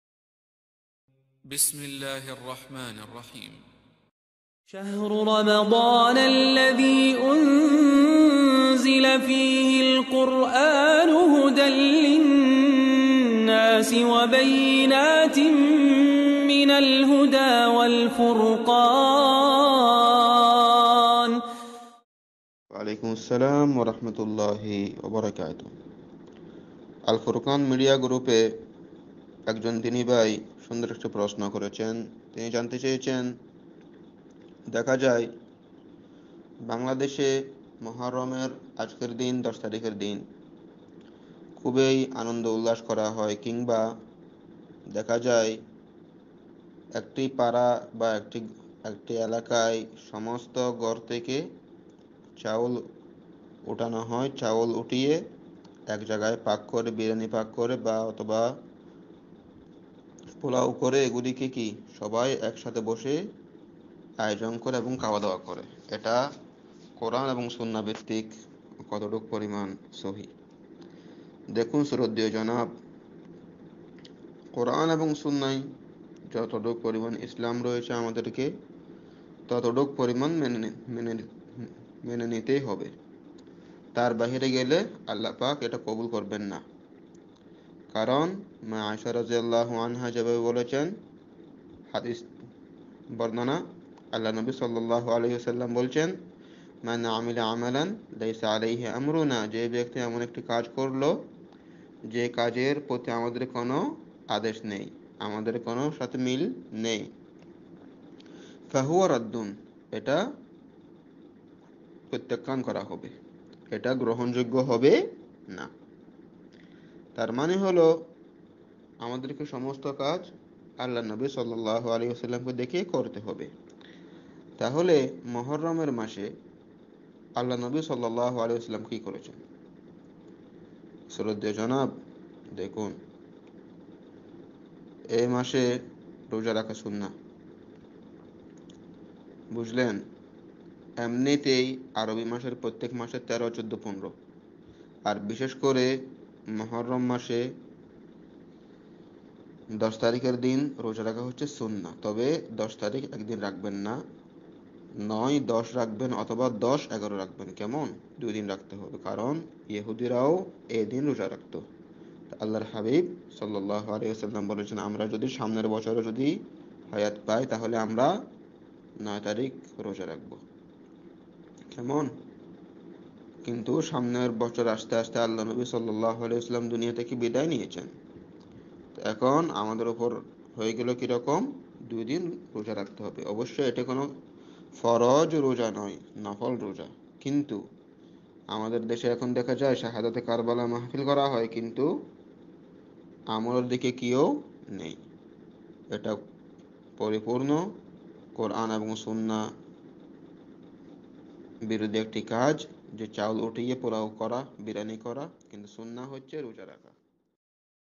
আশুরা উপলক্ষে সম্মিলিত খাবারের আয়োজন করা যাবে কি? আলোচক আহম্মেদ বিন আইয়ুব।
আশুরা উপলক্ষে সম্মিলিত খাবারের আয়োজন করা যাবে কি?  আলোচক আহম্মেদ বিন আইয়ুব। স্বাগতম আপনাকে AL FURQAN media.